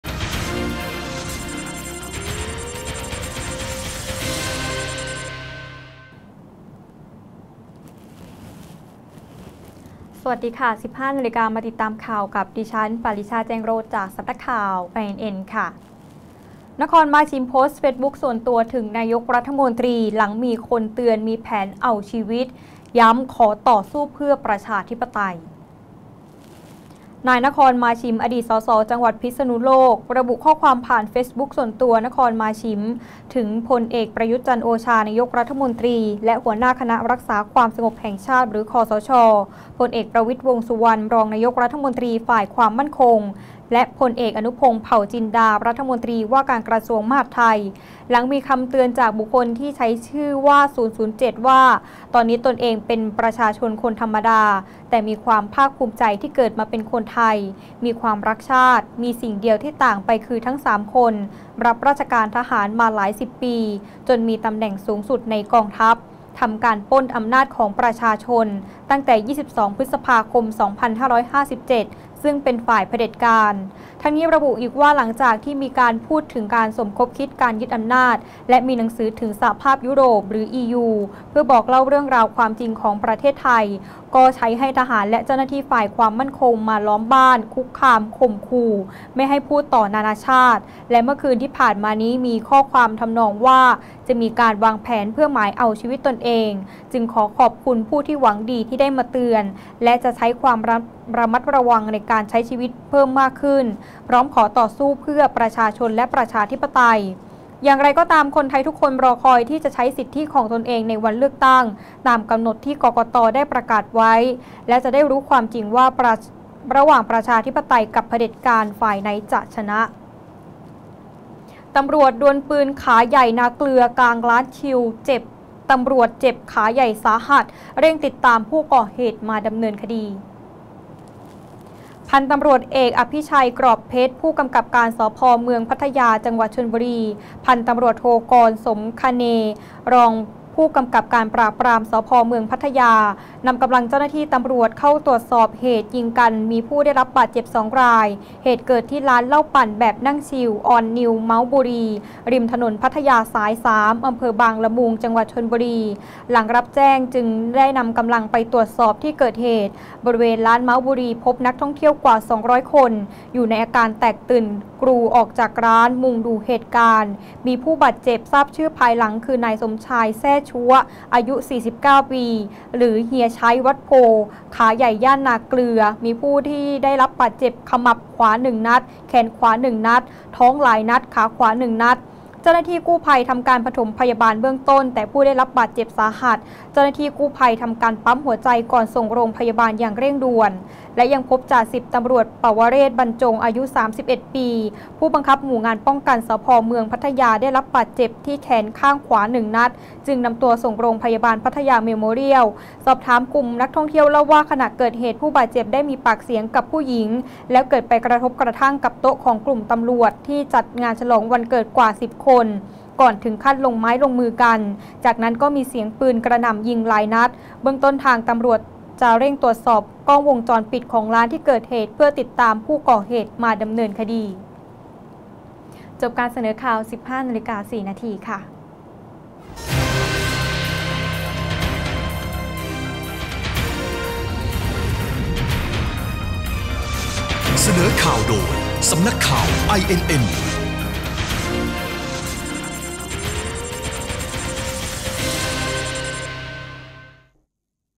ข่าวต้นชั่วโมง 15.00 น.
-"นคร มาฉิม"โพสต์FBส่วนตัวถึงนายกฯ
-ตร.ดวลปืนขาใหญ่นาเกลือกลางร้านชิลชลบุรี